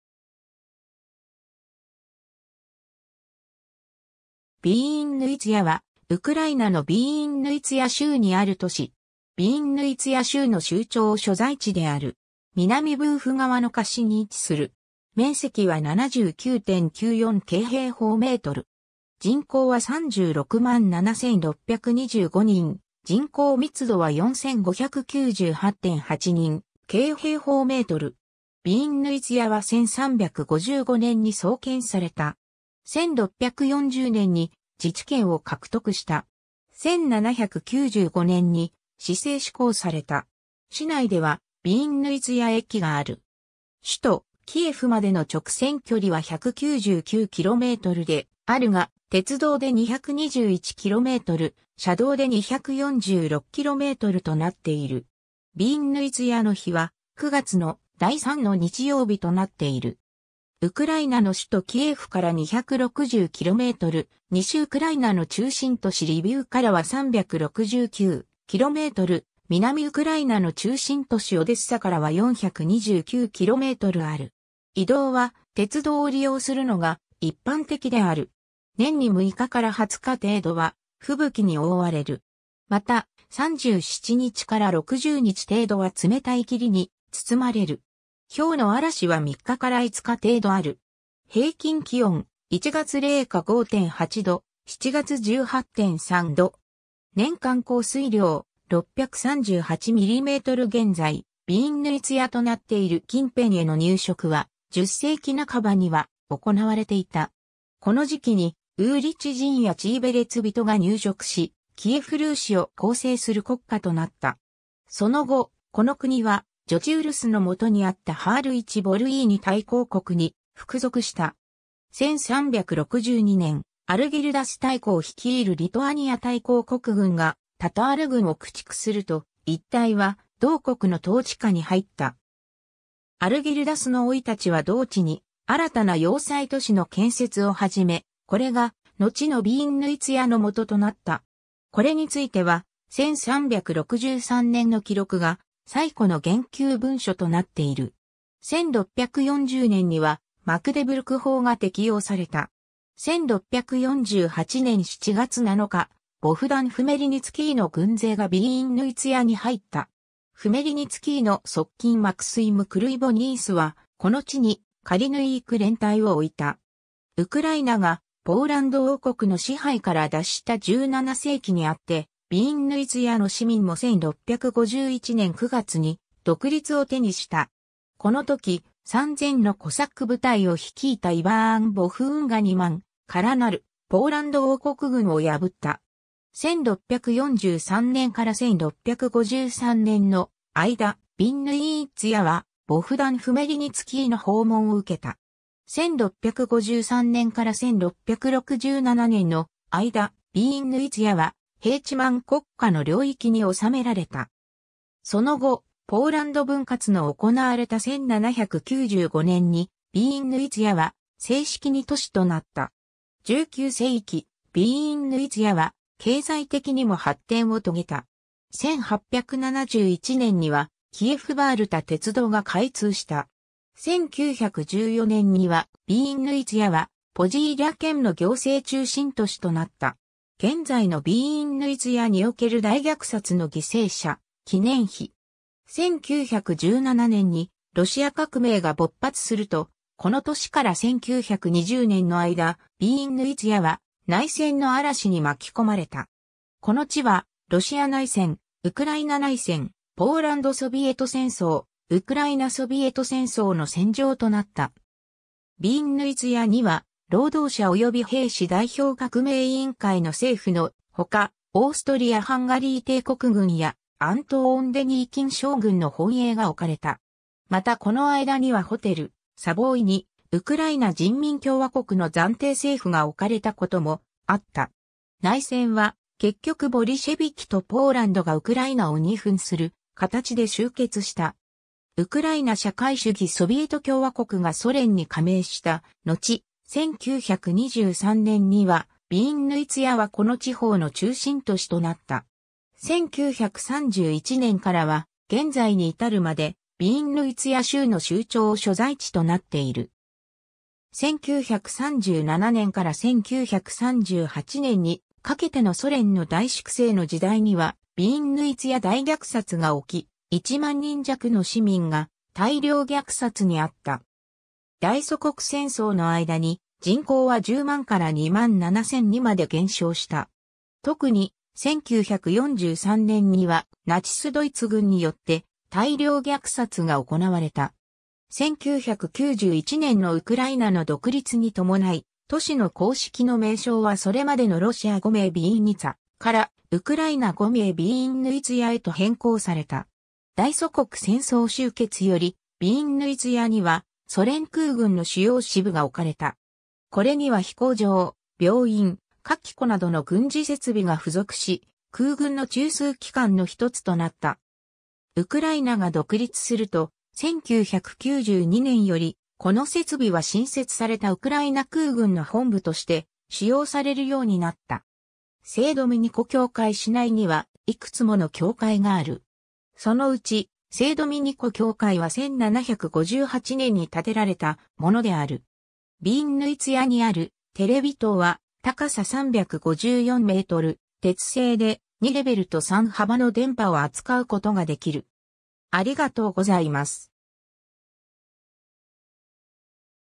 ヴィーンヌィツャ
ヴィーンヌィツャ（ウクライナ語: Вінниця [ˈʋinːɪtsʲɐ] ( 音声ファイル)）はウクライナのヴィーンヌィツャ州にある都市。
ヴィーンヌィツャ州の州庁所在地である。
南ブーフ川の河岸に位置する。
面積は79.94 km²。
人口は367,625人 (2011年8月1日)、人口密度は4598.8 人 / km²。
 ヴィーンヌィツャは1355年に創建された。
1640年に自治権を獲得した。
1795年に市制施行された。
 市内ではヴィーンヌィツャ駅がある。
首都キエフまでの直線距離は199 kmであるが、鉄道で221 km、車道で246kmとなっている。
ヴィーンヌィツャの日は、9月の第3の日曜日となっている。
 ウクライナの首都キエフから260 km、西ウクライナの中心都市リヴィウからは369 km、南ウクライナの中心都市オデッサからは429 kmある。
移動は、鉄道を利用するのが一般的である。
 年に6日から20日程度は、吹雪に覆われる。
また、37日から60日程度は冷たい霧に包まれる。
雹の嵐は3日から5日程度ある。
平均気温：1月 零下5.8 度、7月 18.3 度。
年間降水量：638 mm 現在ヴィーンヌィツャとなっている近辺への入植は、10世紀半ばには行われていた。
この時期にウーリチ人やチーベレツ人が入植し、キエフ・ルーシを構成する国家となった。
その後、この国はジョチ・ウルスのもとにあったハールィチ・ヴォルィーニ大公国に服属した。
 1362年、アルギルダス大公率いるリトアニア大公国軍がタタール軍を駆逐すると、一帯は同国の統治下に入った。
アルギルダスの甥たちは同地に新たな要塞都市の建設を始め、これがのちのヴィーンヌィツャのもととなった。
これについては、1363年の記録が最古の言及文書となっている。
 1640年には、マクデブルク法が適用された。
 1648年7月7日、ボフダン・フメリニツキーの軍勢がヴィーンヌィツャに入った。
フメリニツキーの側近マクスィム・クルィヴォニースは、この地にカリヌィーク連隊(ヴィーンヌィツャ連隊)を置いた。
 ウクライナがポーランド王国の支配から脱した17世紀にあって、ヴィーンヌィツャの市民も1651年9月に独立を手にした。
このとき、3千のコサック部隊を率いたイヴァーン・ボフーンが2万からなるポーランド王国軍を破った。
1643年から1653年の間、ヴィンヌィーツャはボフダン・フメリニツキーの訪問を受けた。
1653年から1667年の間、ヴィーンヌィツャはヘーチマン国家の領域に収められた。
 その後、ポーランド分割の行われた1795年にヴィーンヌィツャは正式に都市となった。
19世紀、ヴィーンヌィツャは経済的にも発展を遂げた。
1871年にはキエフ＝バールタ鉄道が開通した。
1914年には、ヴィーンヌィツャはポヂーリャ県の行政中心都市となった。
 現在のヴィーンヌィツャにおける大虐殺の犠牲者記念碑。
 1917年にロシア革命が勃発すると、この年から1920年の間、ヴィーンヌィツャは内戦の嵐に巻き込まれた。
この地は、ロシア内戦、ウクライナ内戦、ポーランド・ソヴィエト戦争、ウクライナ・ソヴィエト戦争の戦場となった。
ヴィーンヌィツャには、労働者及び兵士代表革命委員会の政府の他、オーストリア・ハンガリー帝国軍やアントーン・デニーキン将軍の本営が置かれた。
またこの間にはホテル「サヴォーイ」にウクライナ人民共和国の暫定政府が置かれたこともあった。
内戦は、結局ボリシェヴィキとポーランドがウクライナを二分する形で終結した。
 ウクライナ社会主義ソヴィエト共和国がソ連に加盟したのち、1923年にはヴィーンヌィツャはこの地方の中心都市となった。
1931年からは、現在に到るまでヴィーンヌィツャ州の州庁所在地となっている。
 1937年から1938年にかけてのソ連の大粛清の時代にはヴィーンヌィツャ大虐殺が起き、1万人弱の市民が大量虐殺に遭った。
 大祖国戦争の間に、人口は10万から2万7千にまで減少した。
特に、1943年にはナチス・ドイツ軍によって大量虐殺が行われた。
 1991年のウクライナの独立に伴い、都市の公式の名称はそれまでのロシア語名ヴィーンニツァからウクライナ語名ヴィーンヌィツャへと変更された。
 大祖国戦争終結より、ヴィーンヌィツャにはソ連空軍の主要支部が置かれた。
これには飛行場、病院、火器庫などの軍事設備が付属し、空軍の中枢機関のひとつとなった。
 ウクライナが独立す...